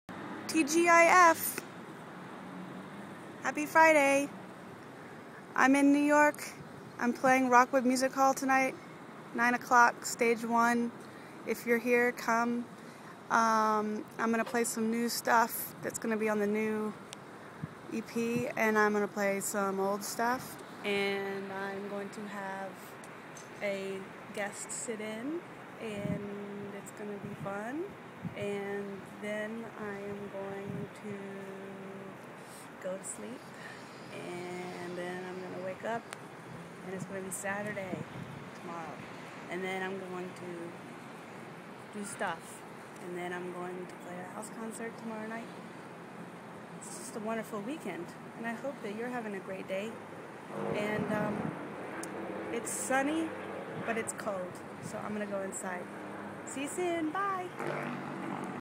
SARAH CAM March 4.MOV
Sarah vlogs from a rooftop on her Spring Swing tour.